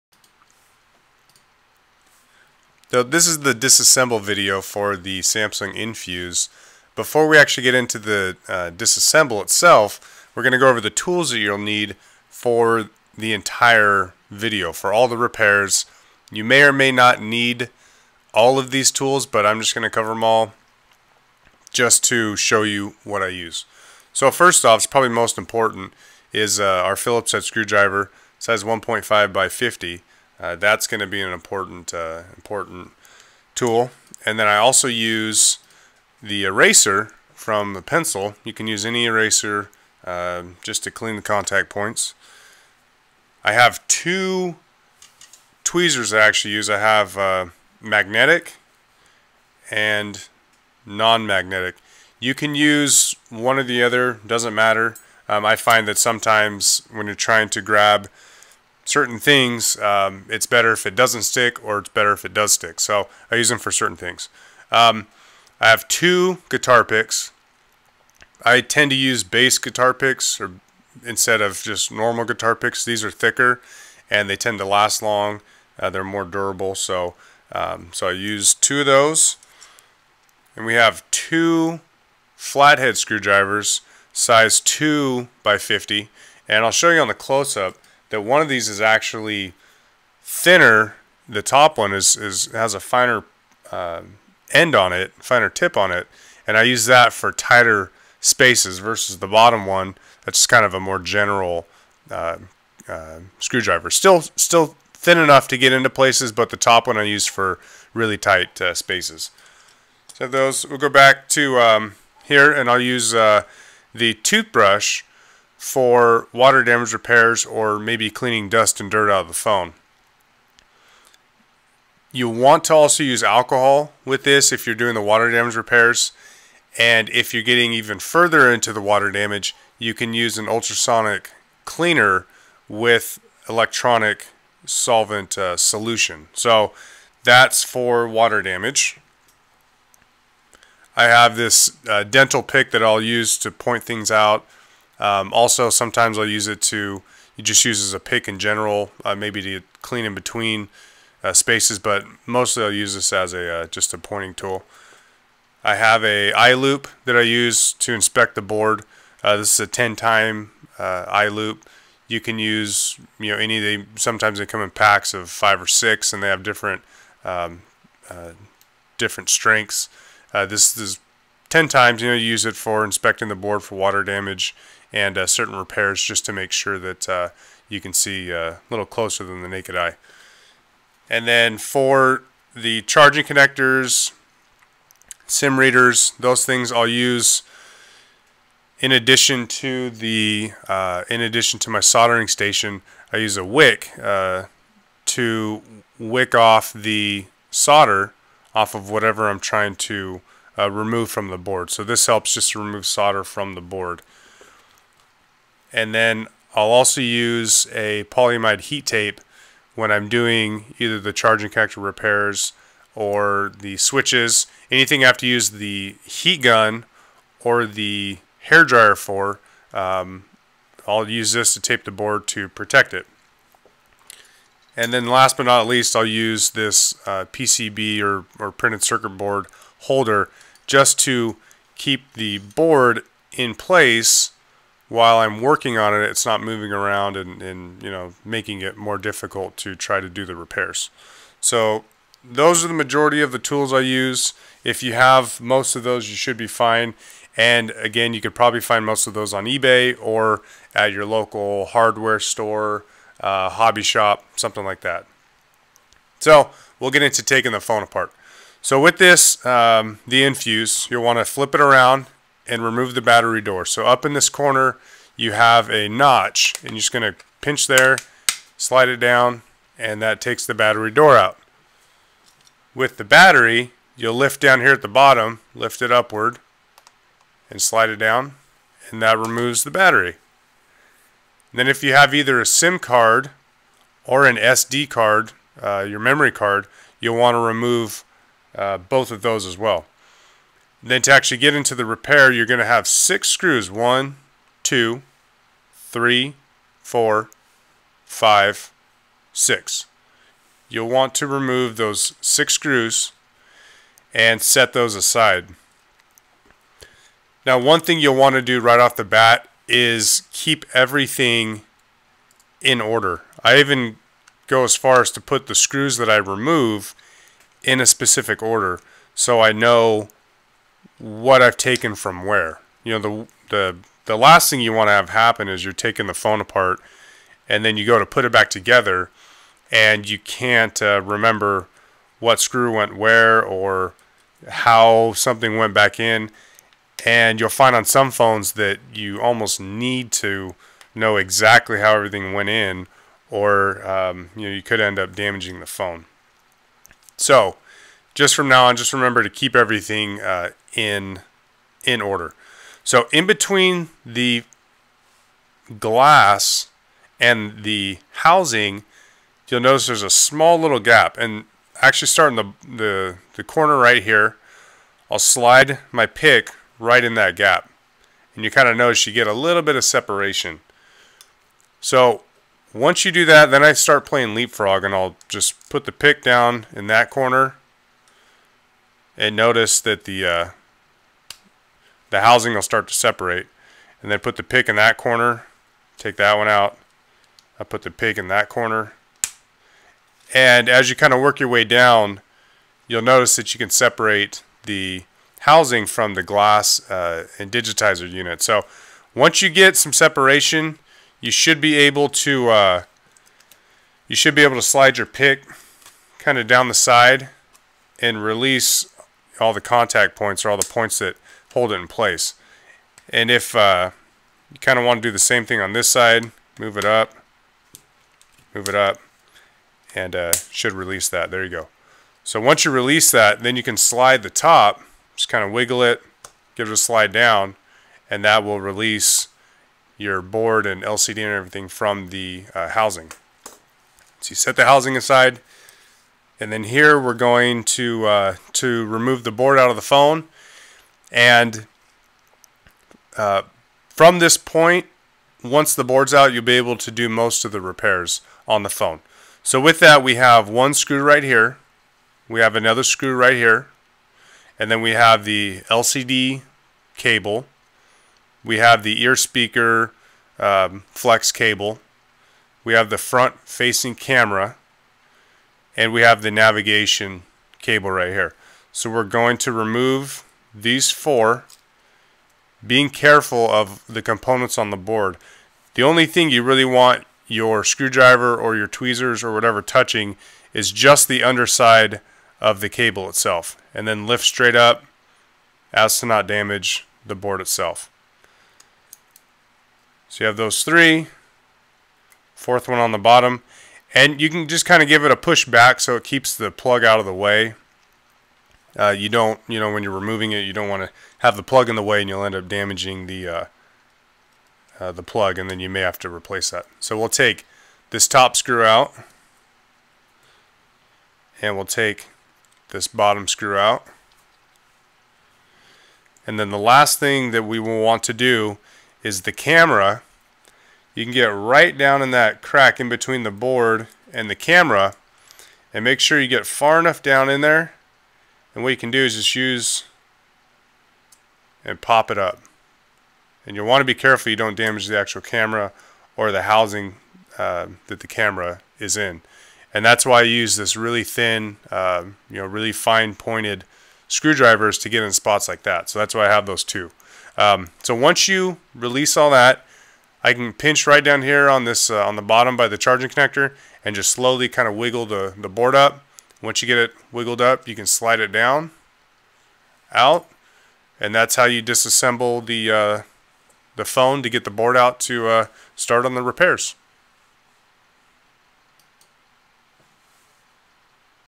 Samsung i997 Disassemble Video. Complete Lesson 1 of 12
GET THE TOOLS YOU NEED RIGHT HERE!!
Screwdriver set - Slotted and Phillips Combo ( 1 Each)

This video covers the tear down of your Samsung i997 Infuse cell phone. We show you the necessary steps to disassemble and get the phone ready for whatever repair you may need to complete.